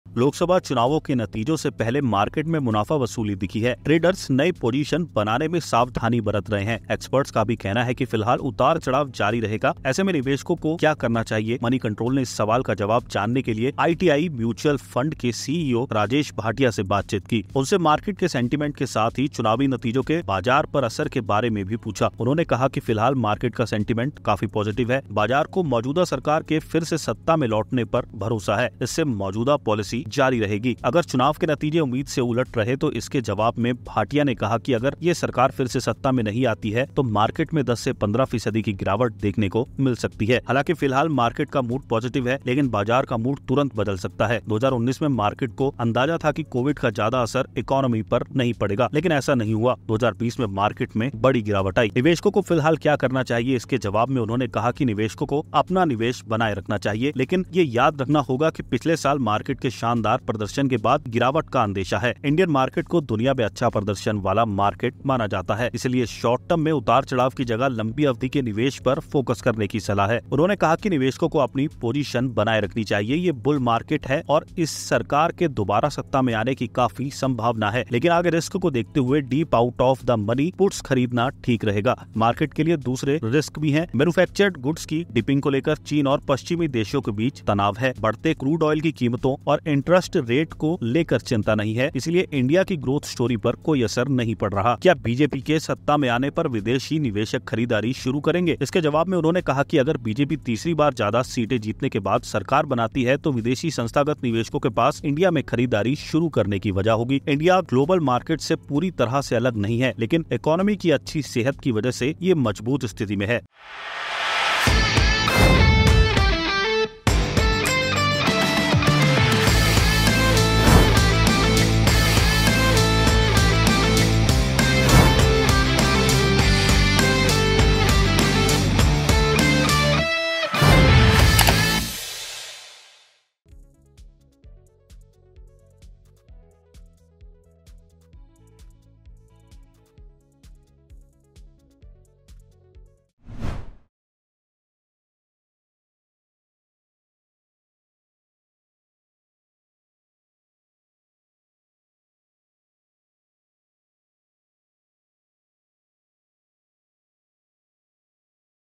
उम्मीद से उलट रहे नतीजे तो 15% टूट सकता है मार्केट
आईटीआई म्यूचअल फंड के सीआईओ राजेश भाटिया ने कहा कि अभी मार्केट का सेंटिमेंट मजबूत है। मार्केट को उम्मीद है कि बीजेपी तीसरी बार केंद्र में सरकार बनाएगी। इससे मौजूदा पॉलिसी जारी रहेगी। ऐसा नहीं होने पर मार्केट में बड़ी गिरावट आ सकती है